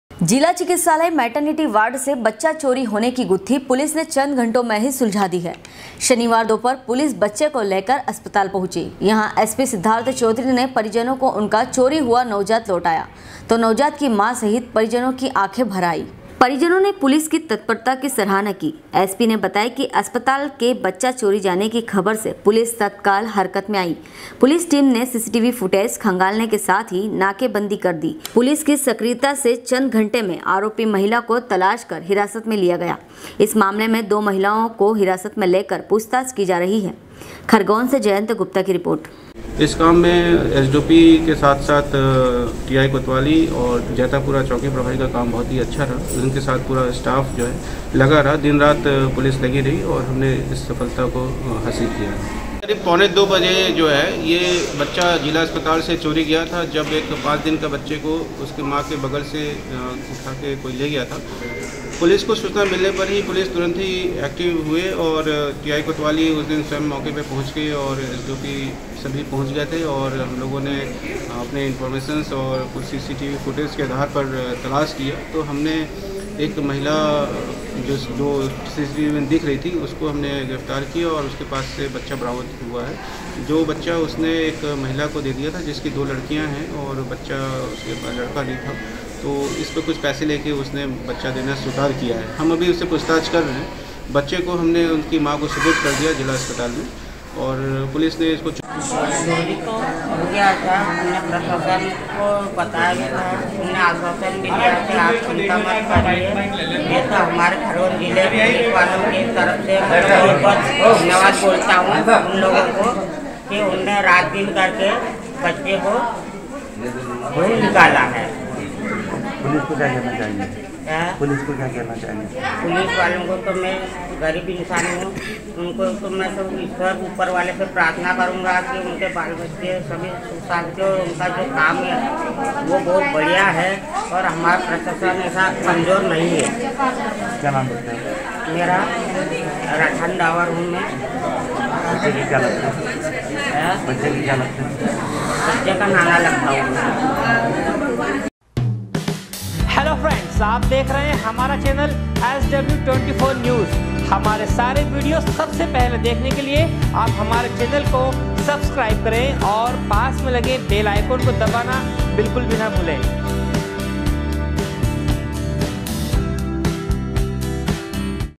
SW 24 News Madhyapradesh
जिला हॉस्पिटल मेटरनिटी वार्ड से बच्चा चोरी होने की गुत्थी पुलिस ने चंद घण्टो में ही सुलझा दी है। शनिवार सुबह पुलिस बच्चे को लेकर अस्पताल पहुची यहां एसपी सिद्धार्थ चौधरी ने परिजनों को उनका चोरी गया नवजात लौटाया तो नवजात की माँ सहित परिजनों की आंखे भर आई। खरगोन से जयंत गुप्ता की रिपोर्ट